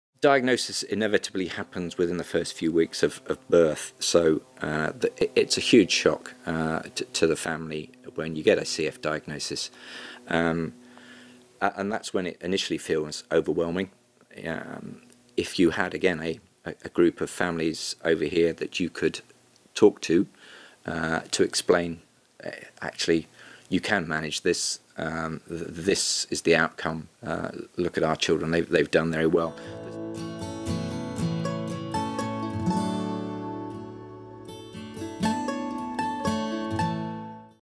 Hidden Disabilities: Cystic Fibrosis - Diagnosis (Twitter)
Cystic Fibrosis is a life limiting inherited genetic condition caused by a faulty gene. The faulty gene controls the movement of salt and water in and out of cells.

This video was produced by On Screen Creations. Find out about their video services by visiting